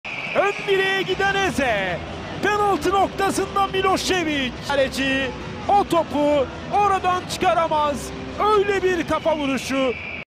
هدف قونيا سبور الثاني في فيتوريا غيماريش| Konyaspor - Vitória Guimarães
Deni Milošević